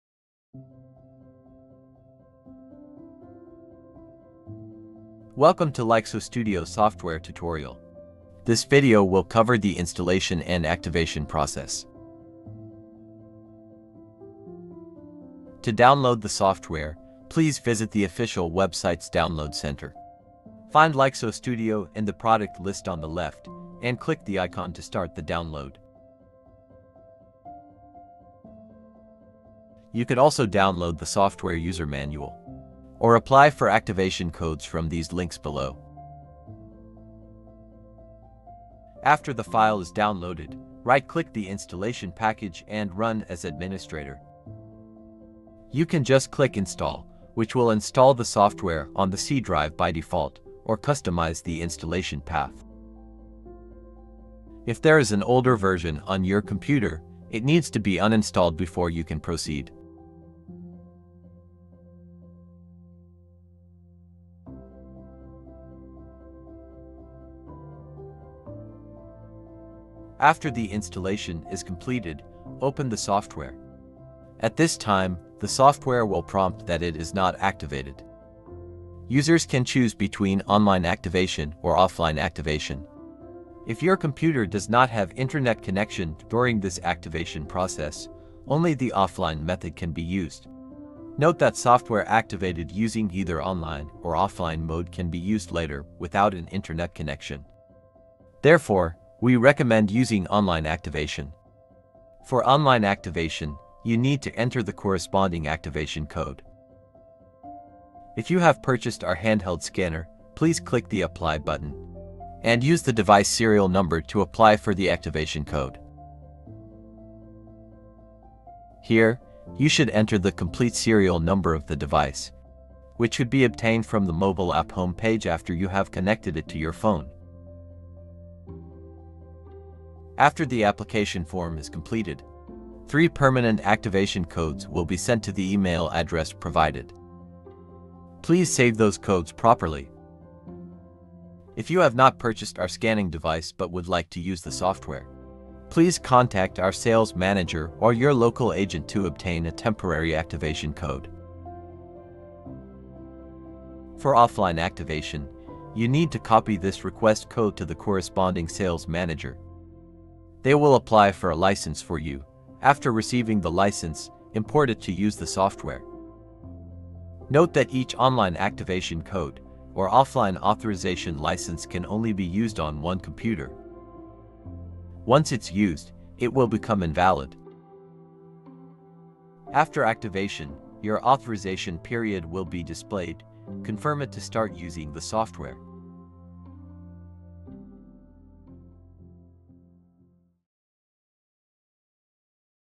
LixelStudio 3.0 Tutorial - Episode 1: Installation and Activation
Welcome to the LixelStudio 3.0 tutorial series! In this first episode, we guide you step-by-step through the installation and activation process of LixelStudio 3.0. Learn how to set up the software smoothly so you can get started on your 3D modeling journey.